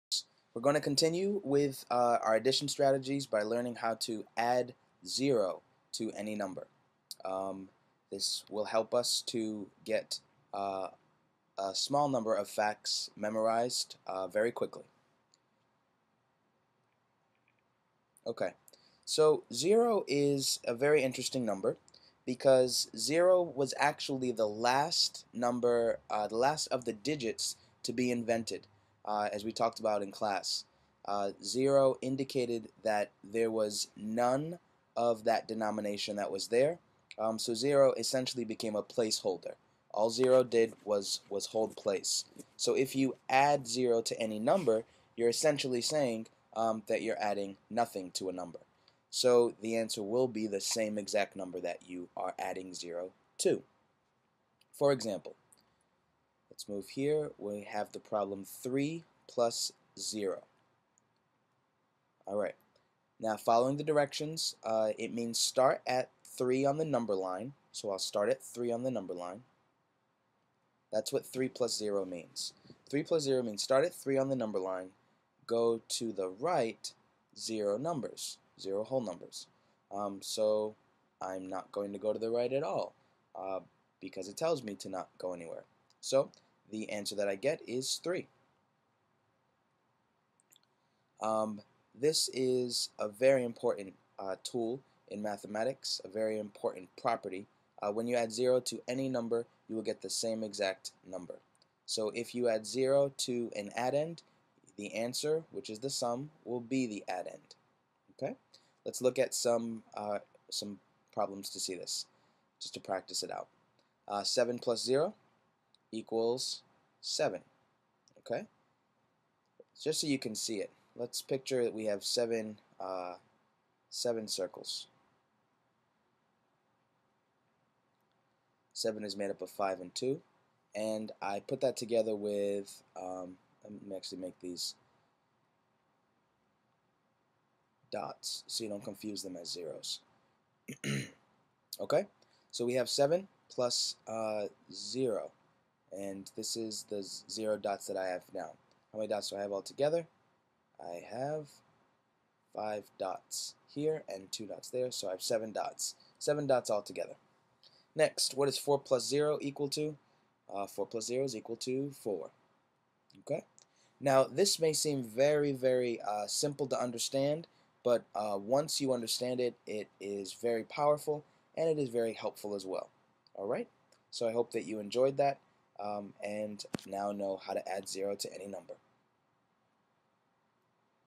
K.OA.5 / 1.OA.3 - Quickly Learn Basic Addition Facts: Add 0
Mr. Almeida explains how to add 0 to any number and why the sum is the way it is using the number line.  This basic fact helps to support the Common Core math standards of K.OA.5 and 1.OA.3.  This simple fact is known as the additive identity property or the identity property of addition, which states that when you add 0 to any number, you get a sum of the identical addend to which you added 0.